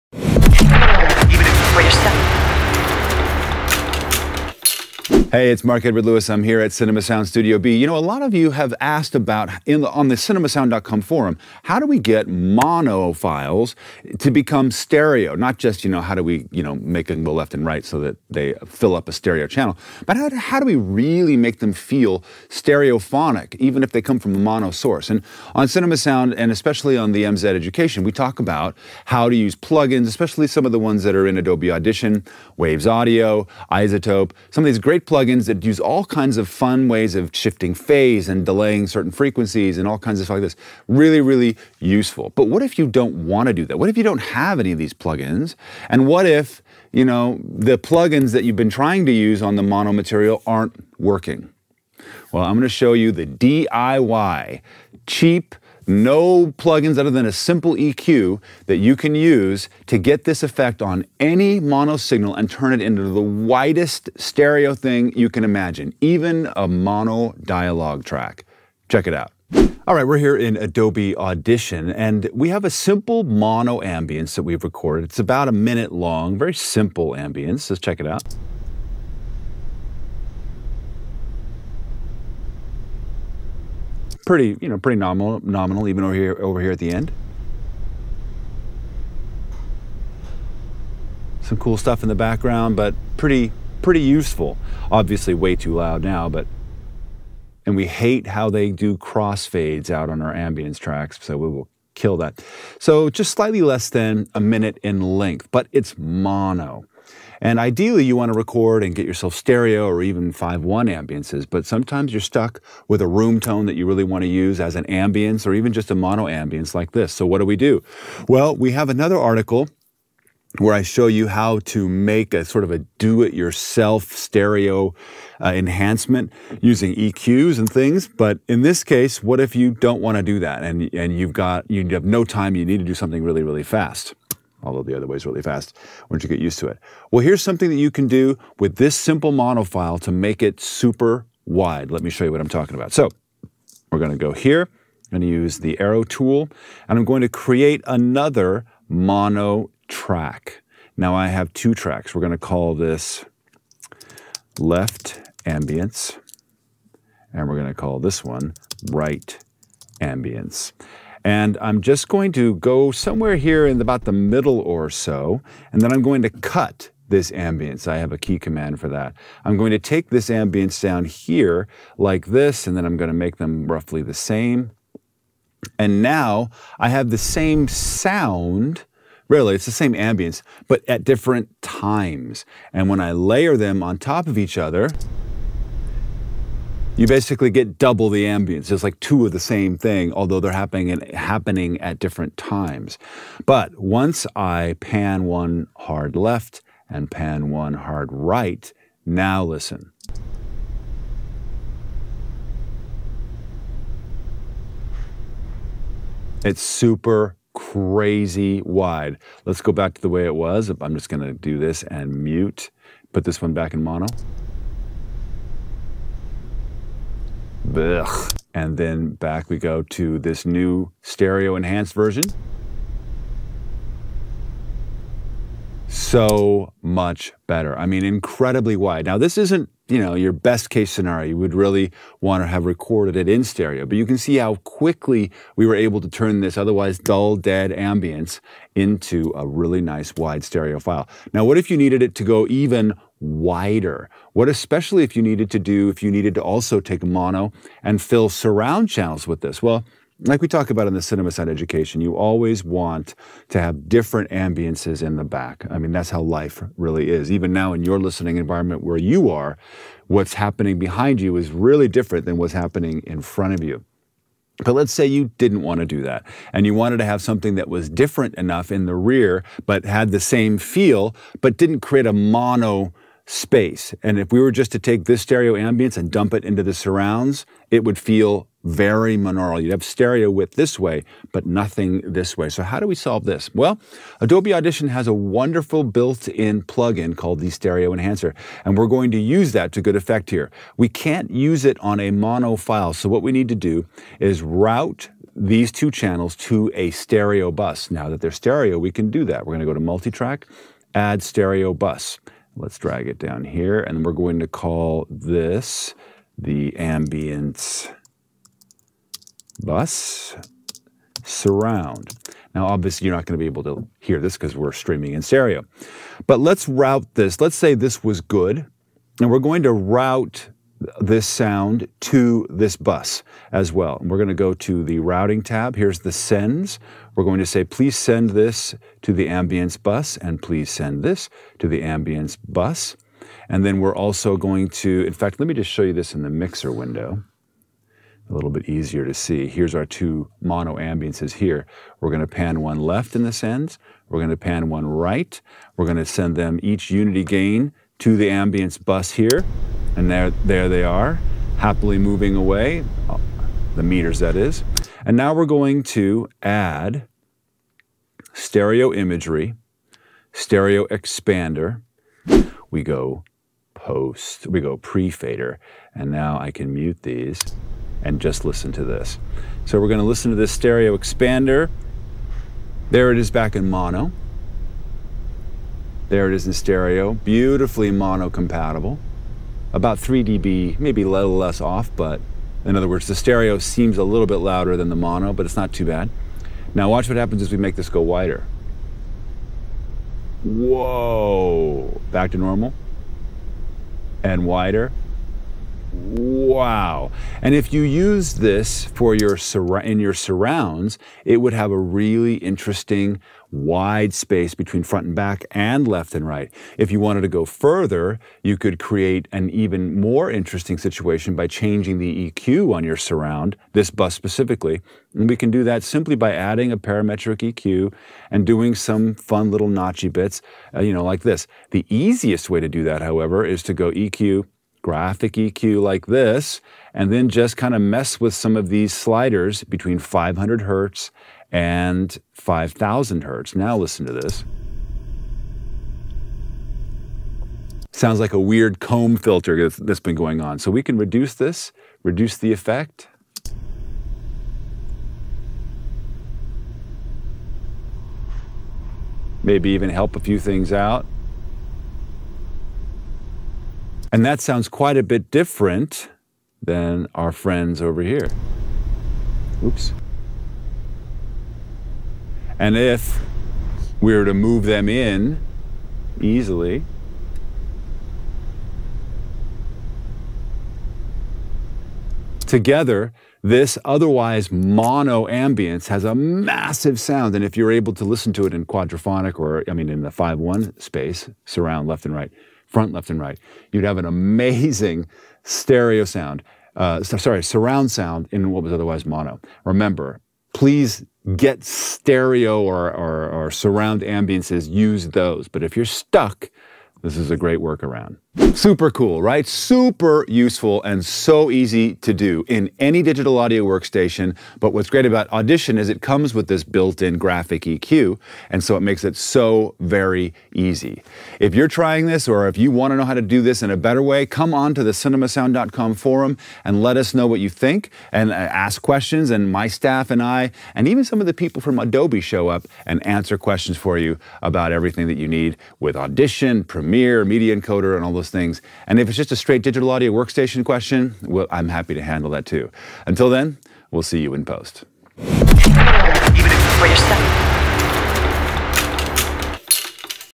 DIY Mono Ambiences to Stereo in Adobe Audition with Only EQ Plugins
Get Adobe Creative Cloud (and Audition and Premiere Pro) Here!

In this clip, audio guru Mark Edward Lewis from Cinema Sound shows us how to create wide stereo Ambiences from mono Ambiences clips - without ANY PLUGINS but a simple EQ in Adobe Audition - this can work in. ANY DAW.

Learn more about how to get a Hollywood $50 million impact on your audience